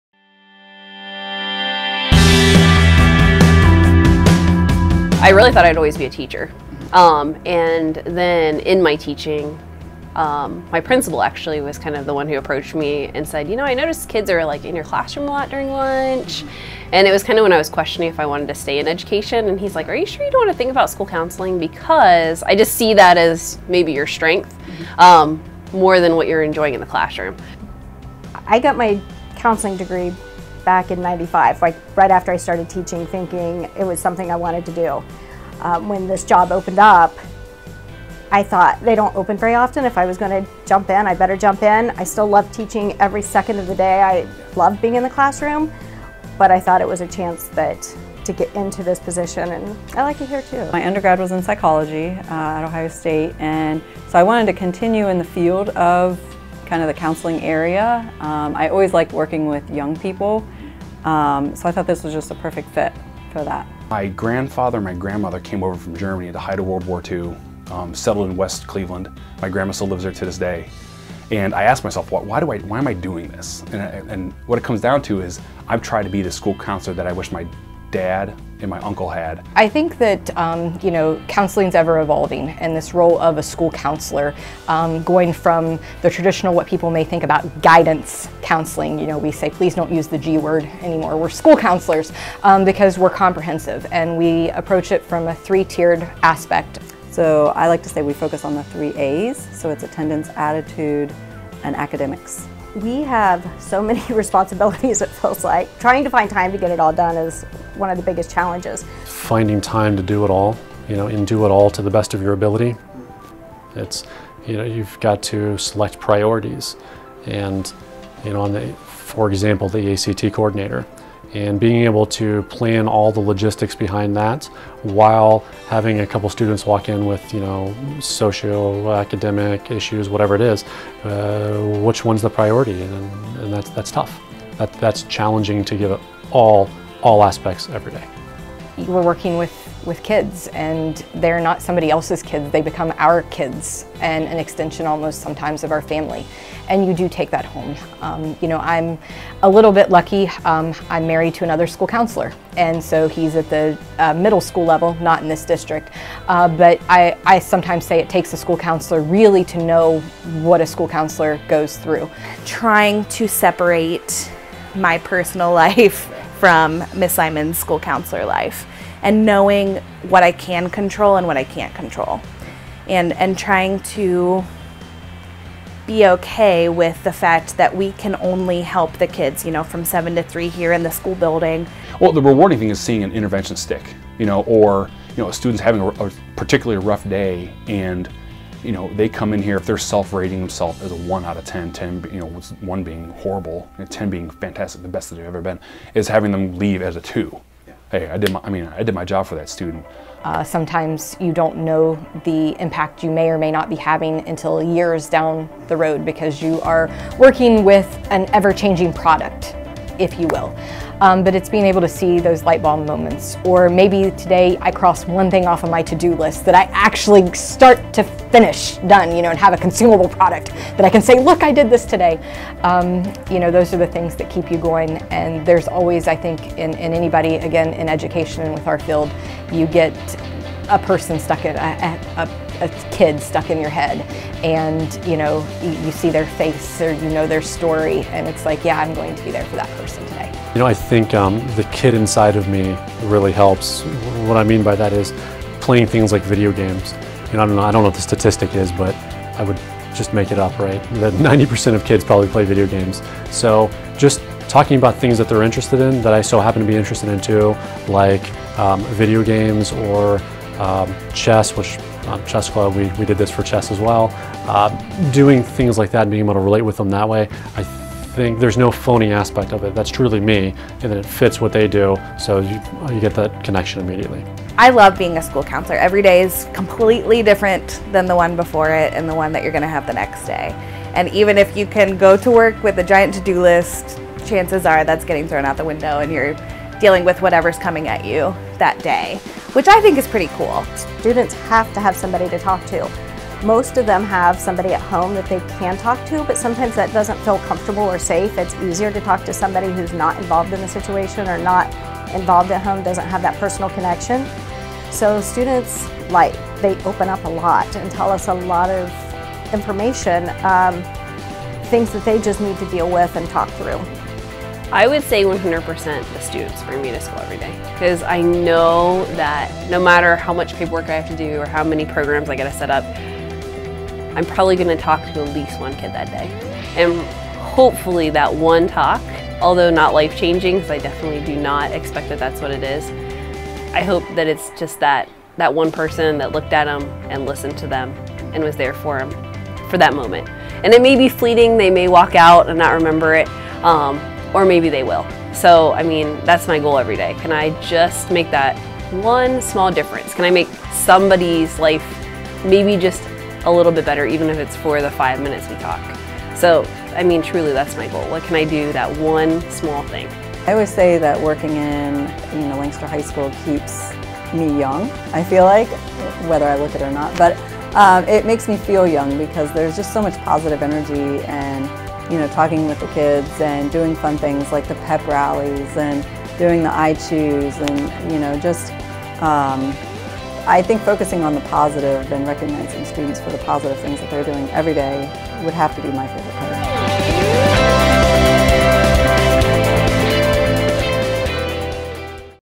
In Appreciation Of Our School Counselors...
For many students at Lancaster City Schools, the school counselors represent a pathway to success, a caring ear, a good laugh, or just a friendly face in the hallways. These superstar counselors give 100% for their schools and we recognize their efforts by letting you hear their stories here